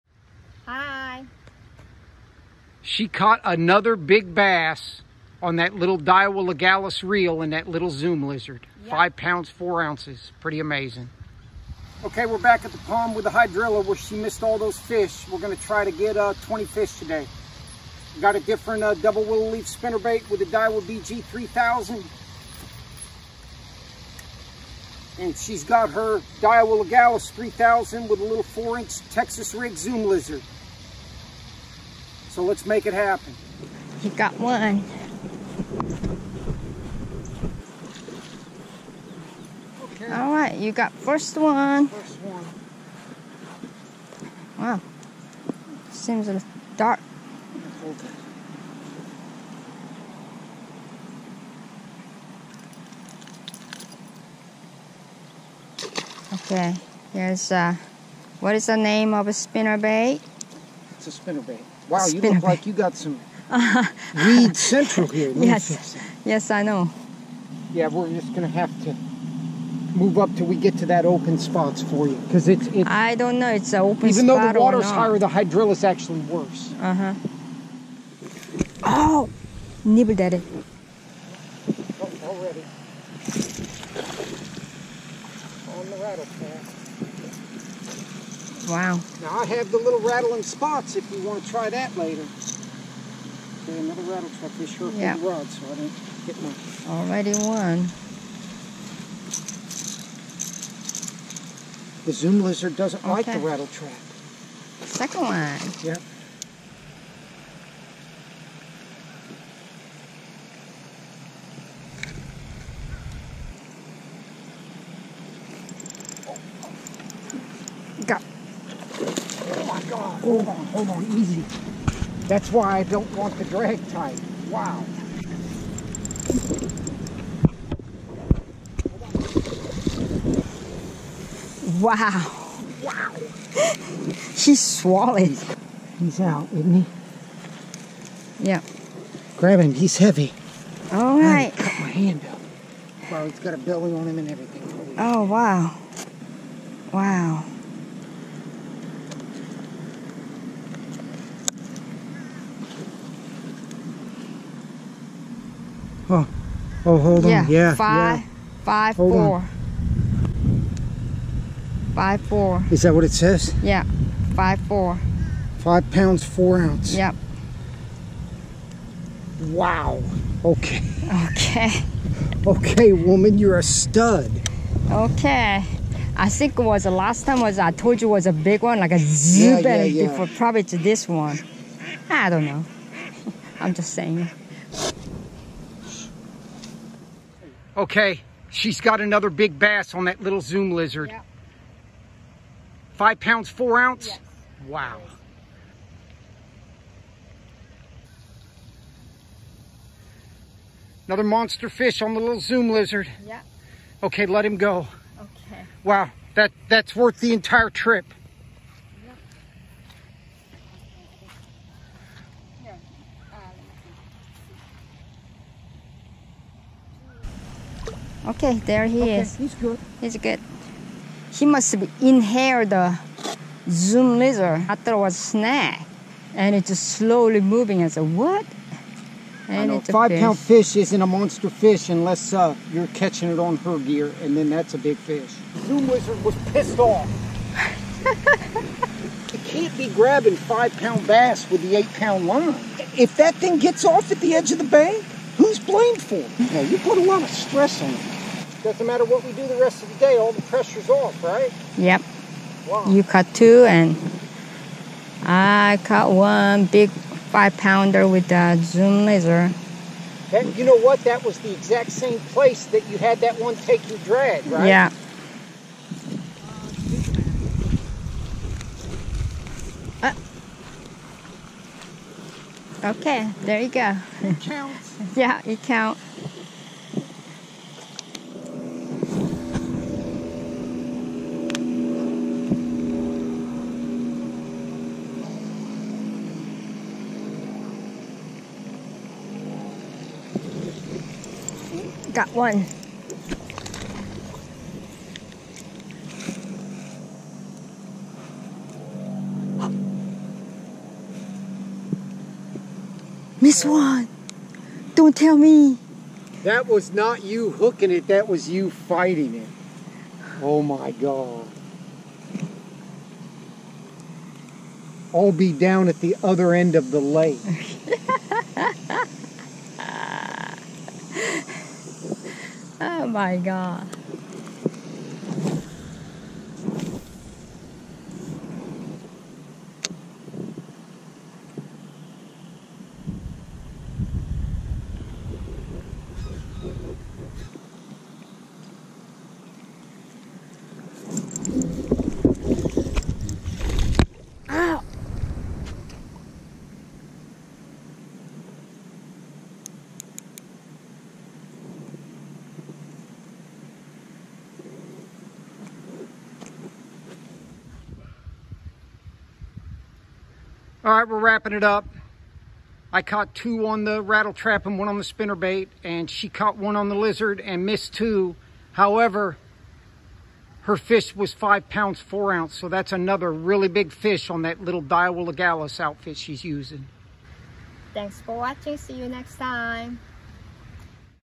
Daiwa Legalis LT 3000 catching another BIG BASS!!
Daiwa Legalis LT 3000 catching another BIG BASS!! My wife caught her 2nd big bass over 5lbs on her
little Daiwa Legalis LT 3000 and the 4 inch ZOOM LIZARD!! She is only using 8lb line and has caught
about 12 bass over 3lb including this 5lb 4oz and her PB 6lb 2oz all on the 4 inch zoom lizard!! She
is approaching 100 bass on the Daiwa Legalis LT 3000!!

ROD - Gander MTN vortex 6'6" medium light action

LINE - Berkley trilene 8lb XL

LURE - 4 inch ZOOM LIZARD ( black/red flake )